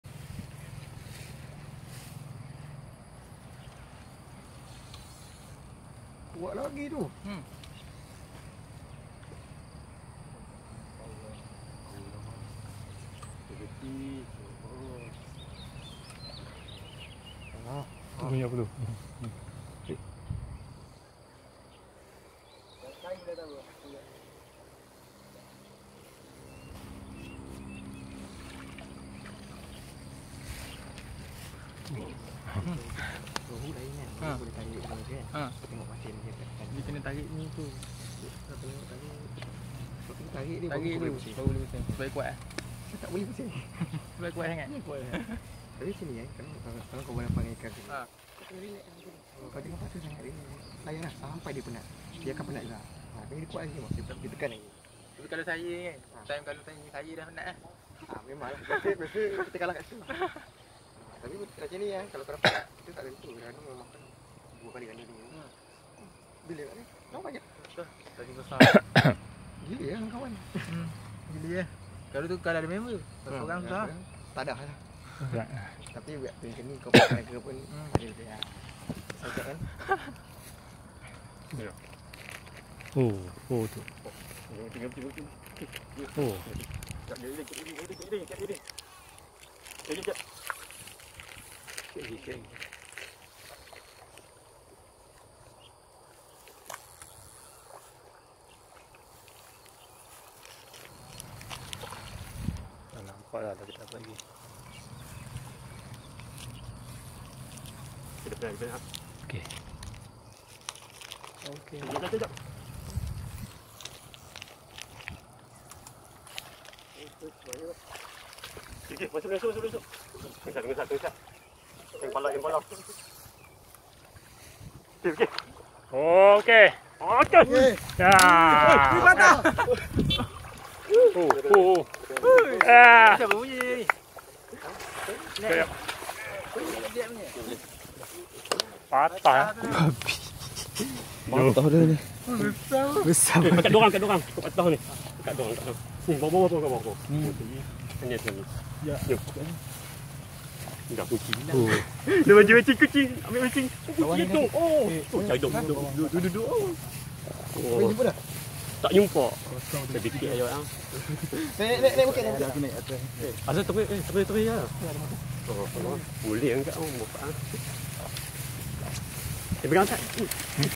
MEMBURU PATIN KOLAM UPM (PART 2)
Patin 7kg berjaya ditawan  tapi ada lagi besar